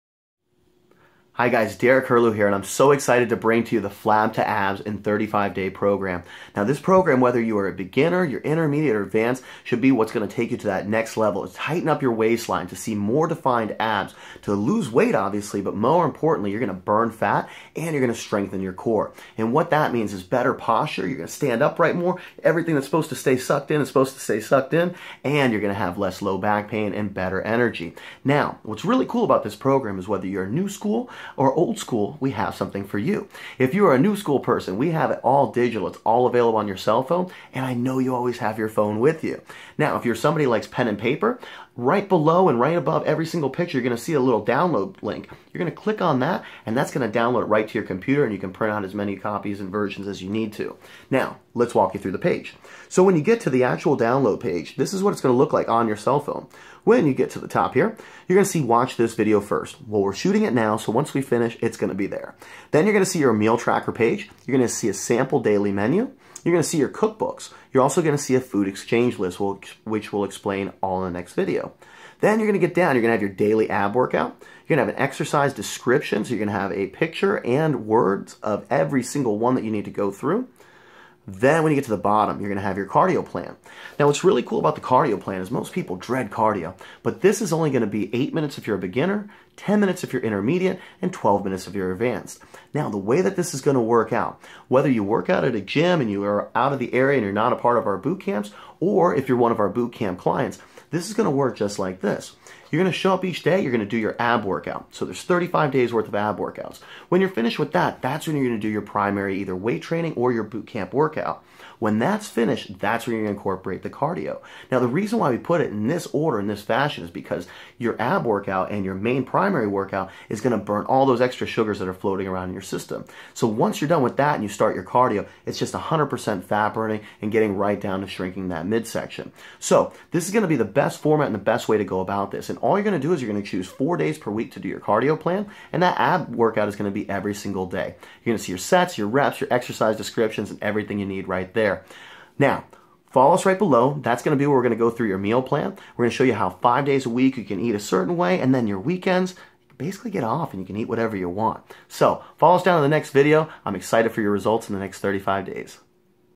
Flab2Abs in 35 days
LiveFitTraining.co/flab-to-abs learn about this complete 35 day program that includes an easy-to-follow meal plan, daily ab workout and weekly cardio routine along with a smoothie recipe book with a hundred different options, food exchange list, cookbook and much more!